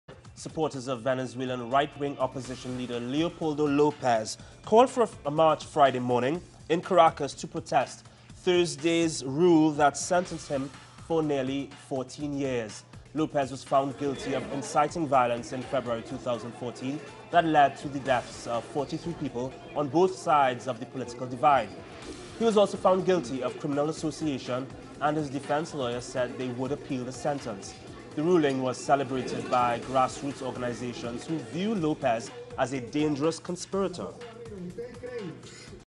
Venezuela: Supporters Rally to Jailed Rightist Leader
Supporters of jailed far-right Venezuelan opposition leader Leopoldo Lopez rallied in Caracas on Friday to protest his 14 year prison sentence. Lopez is accused of having instigated the violence that led to 43 deaths in February 2014 protests. Grass roots movements, however, hailed the court ruling, as they view Lopez as a dangerous and violent conspirator. Lopez's lawyers plan to appeal the sentence. teleSUR